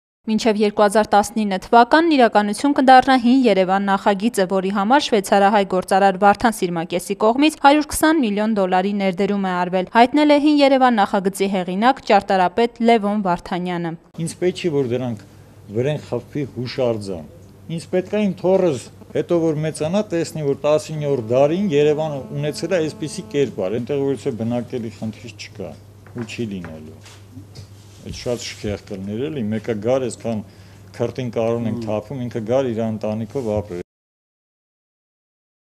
Մինչև 2019-ը կտեսնենք «Հին Երևան»-ը
«Հին Երևան» նախագիծը կենդանության նշույլներ է ցույց տալիս։ Նախագծի հեղինակ, ճարտարապետ Լևոն Վարդանյանը լրագրողներին տեղեկացրել է, որ մինչև 2019 թվականն իր չափագրած բոլոր շենքերը վերականգնված կլինեն և նախախորհրդային շրջանի Երևանի սիրահարները հնարավորություն կունենան վայելելու այդ շրջանի ճարտարապետությունը։ «Ազգային, բնակելի հրաշալի տներ են եղել, որ հիմա չկան, պետք է վերականգնենք»,-շեշտել է ճարտարապետը։

Նրա խոսքով՝ վերջին տարիներին մայրաքաղաքում ոչ մի շենք չի ապամոնտաժվել, բոլորը քանդվել են, բացառությամբ Տերյան 11 հասցեի։ Իսկ վերականգնվող շենքերը հավանաբար կծառայեն որպես թանգարաններ։ Պարոն Վարդանյանի խոսքով՝ հիմա Երևանում այնպիսի շենքեր են կառուցվում, որ չես հասկանում իսպանացու, իտալացու, թե մեկ այլ ազգի մարդկանց տներ են։ Ասում է՝ Երևանում ու Թիֆլիսում Հուշարձանների բաժին միաժամանակ է բացվել, բայց այնտեղ կարողացան պահպանել հինն, ի տարբերություն մեզ։